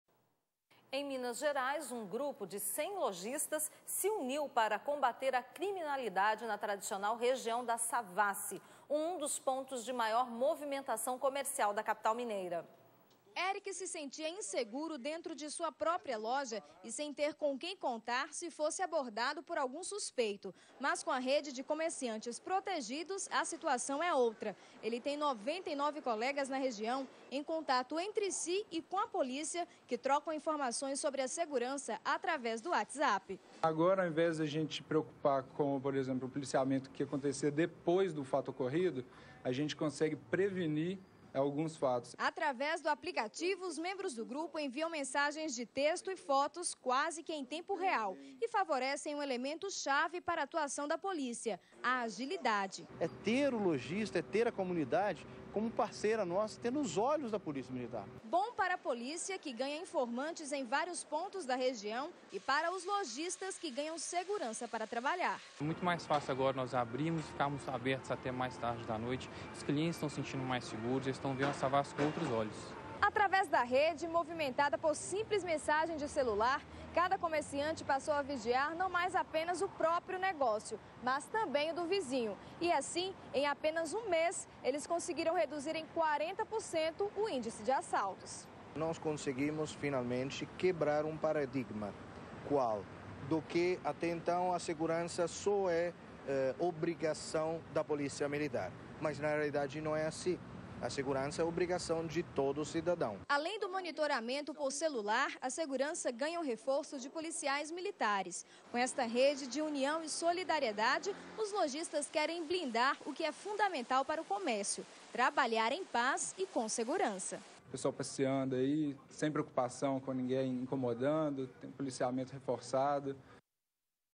Lojistas de BH formam grupo no watsapp para combater criminalidade - CN Notícias
Em Minas Gerais, um grupo de cem lojistas se uniu para combater a criminalidade na tradicional Região da Savassi, um dos pontos de
maior movimentação comercial da capital mineira.

Reportagem de Catarina Jatobá